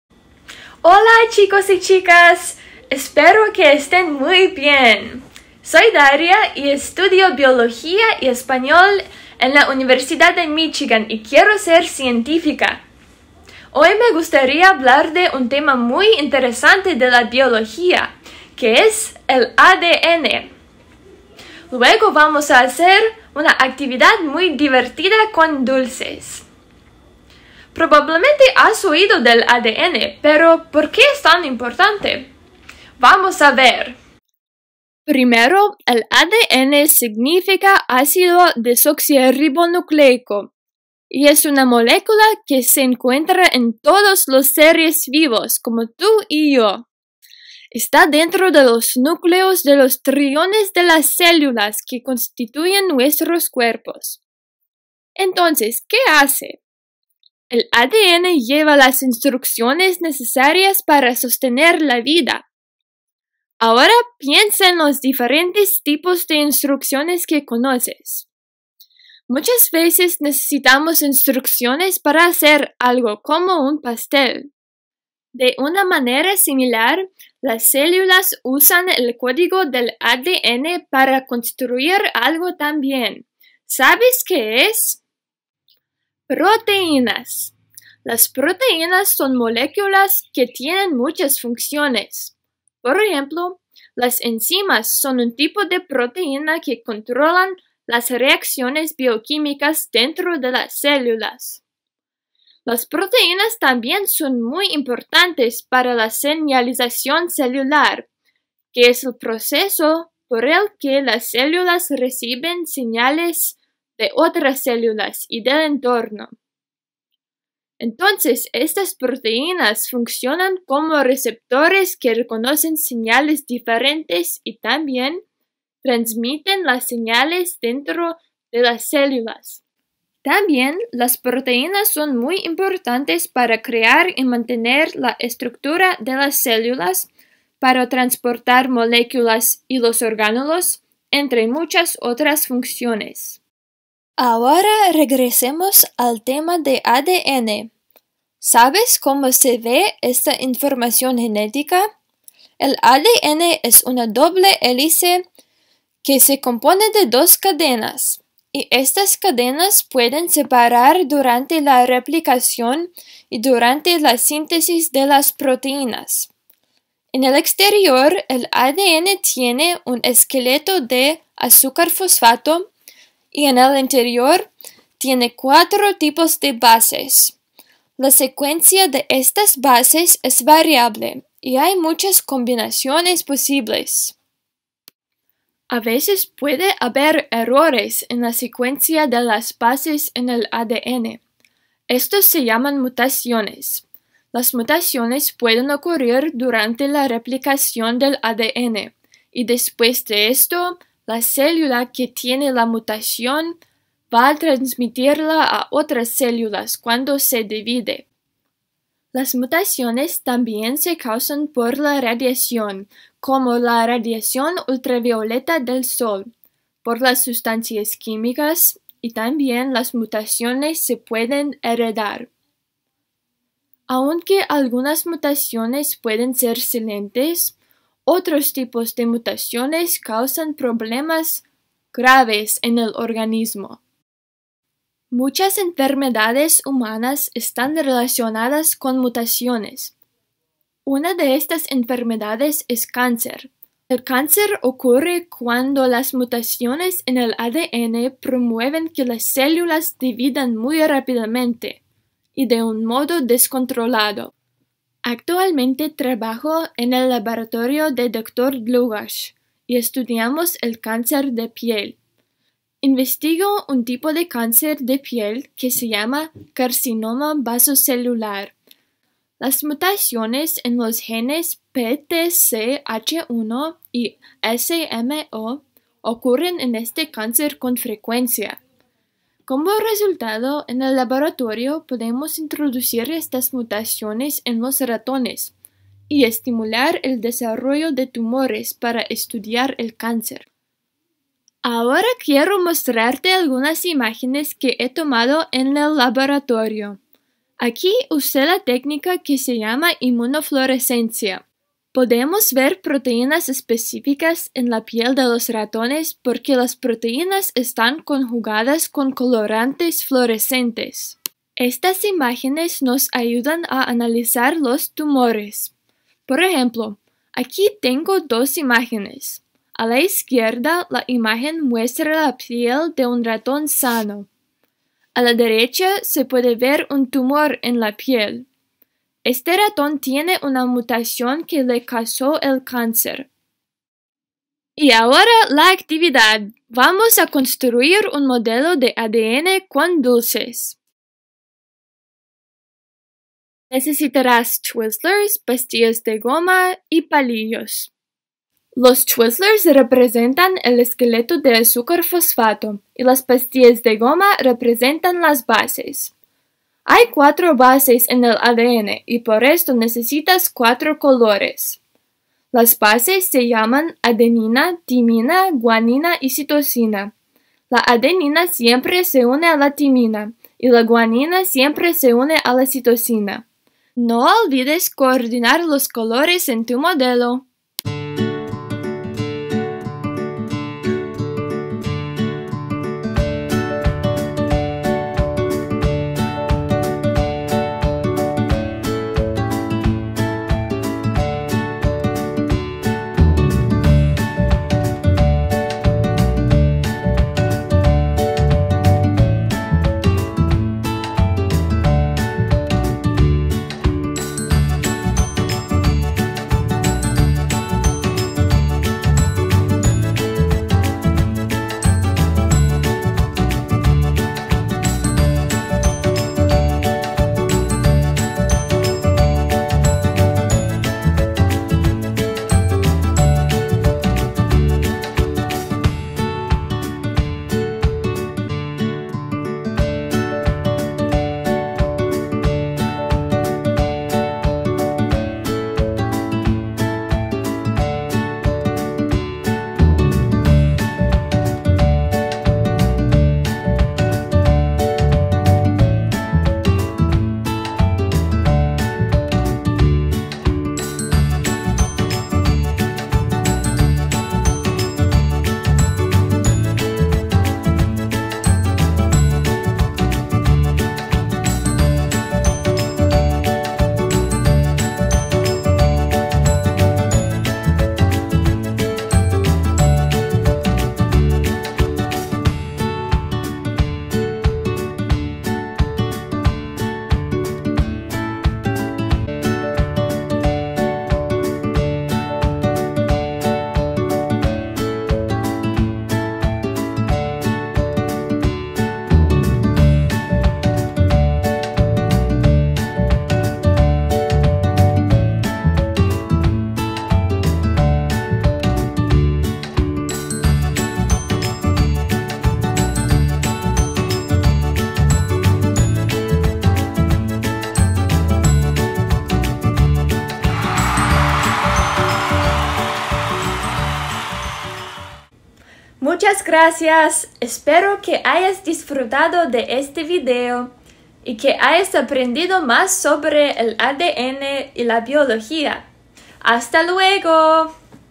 Undergrad Science - DNA | Daria Pyrozhenko
Desde su descubrimiento en 1953, la estructura de doble hélice del ADN se ha vuelto icónica. Esa estructura es lo que permite que el ADN funcione. ¡Daria, una estudiante de biología en U-M, explica en español! Bono: ¡Hay dulces!

Since its discovery in 1953 double helix structure of DNA has become iconic. That structure is what allows DNA to function. U-M biology student Daria explains in Spanish! Bonus: There's candy involved! English subtitles included.